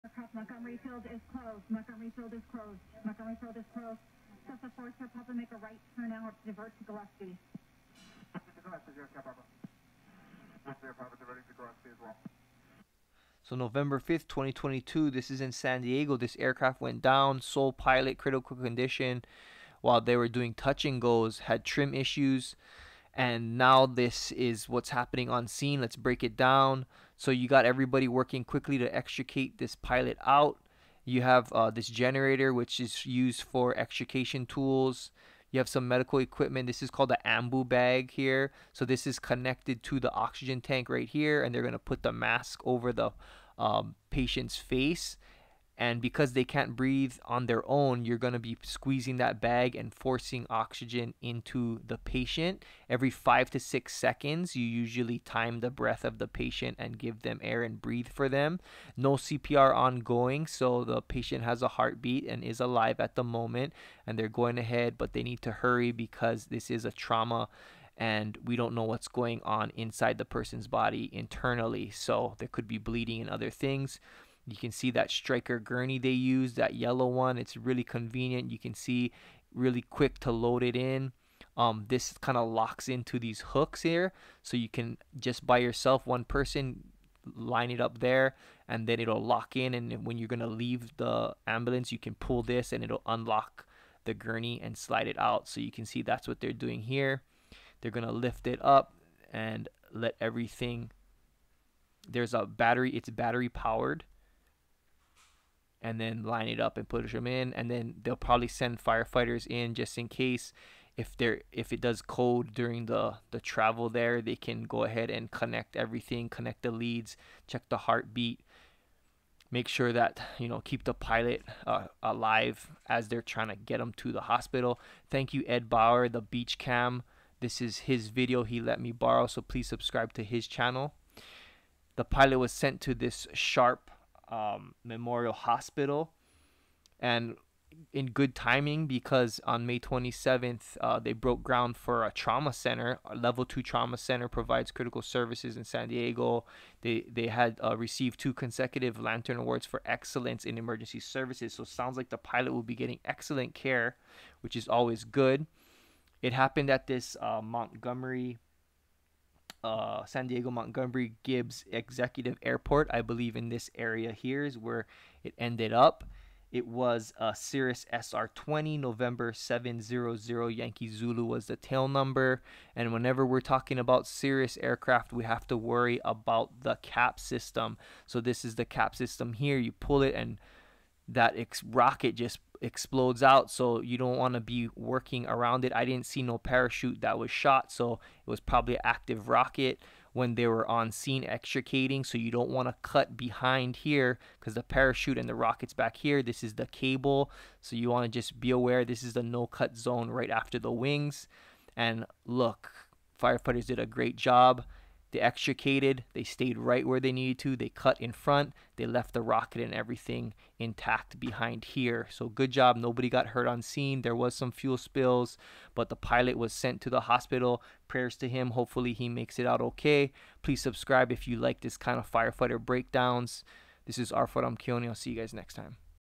Watch as Firefighters Save Cirrus Pilot in San Diego
A Cirrus SR-20 (N700YZ) pilot had Trim issues and crashed at Montgomery-Gibbs Executive Airport in San Diego on November 5, 2022.  The pilot was in critical condition and sent to Sharp Memorial Hospital.